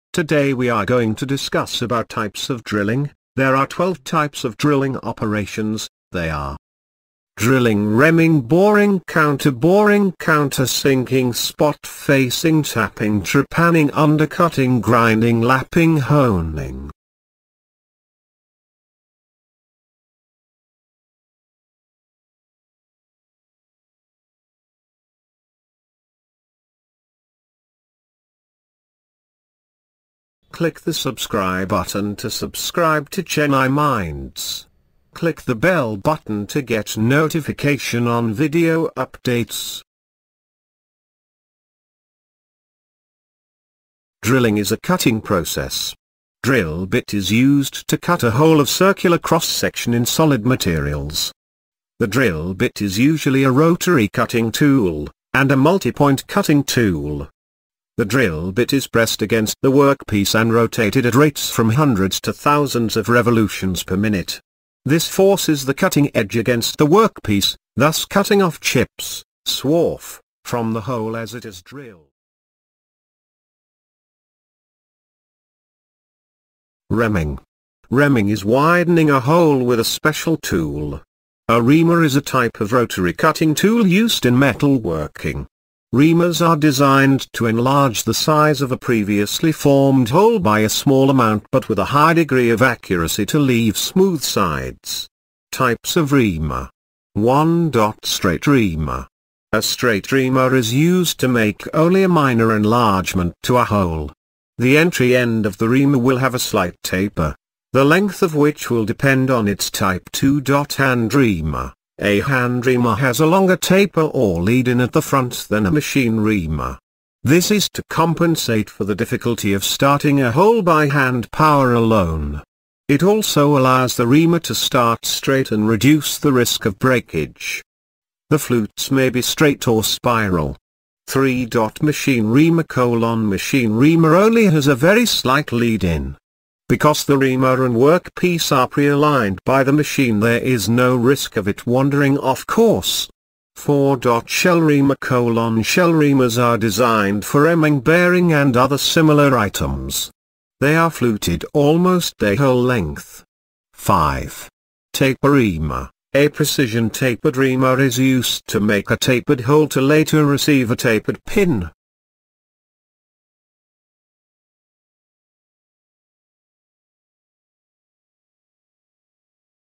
DRILLING AND REAMING|PART-1|DRILLING TYPES
we're on Instagram as @chennaiminds. Install the app to follow my photos and videos.

Drilling is a cutting process
drill bit is used to cut a hole of circular cross-section in solid materials.
The drill bit is usually a rotary cutting tool,and a multipoint cutting tool.
The drill bit is pressed against the workpiece and rotated at rates from hundreds to thousands of revolutions per minute.
This forces the cutting edge against the workpiece, thus cutting off chips (swarf) from the hole as it is drilled.